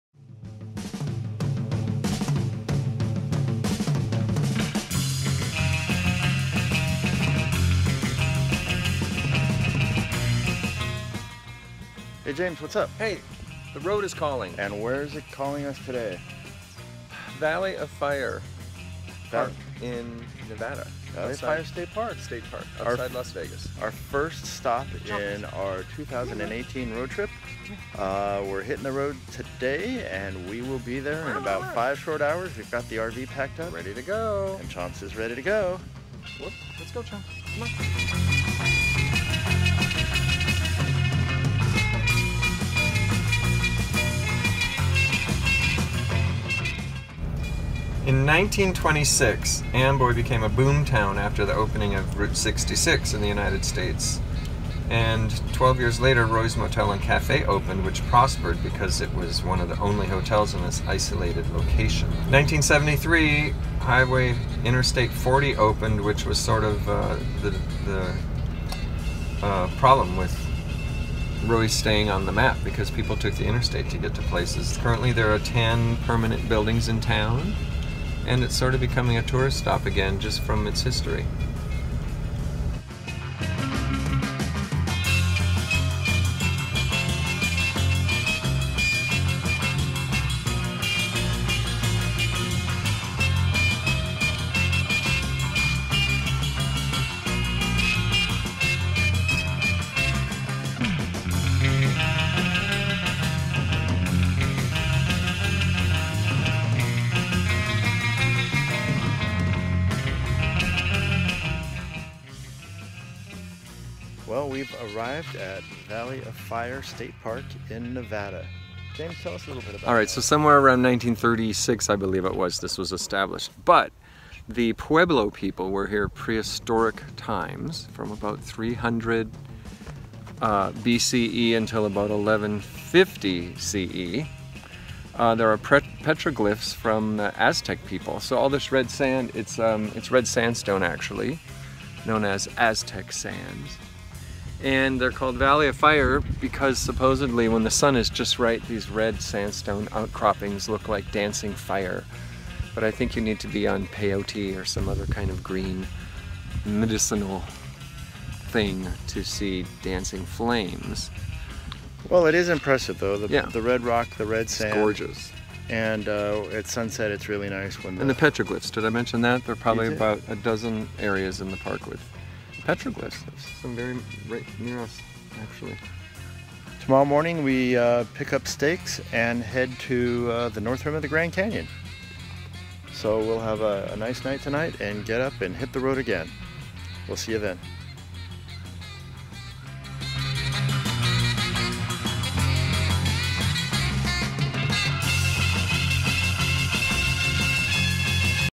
AMBOY, CALIFORNIA | VALLEY OF FIRE STATE PARK, NEVADA
On the first leg of this years road trip we stop at Amboy, California and set up camp at Valley Of Fire State Park in Nevada.

Our Amazon RV Store:

GayRVing.com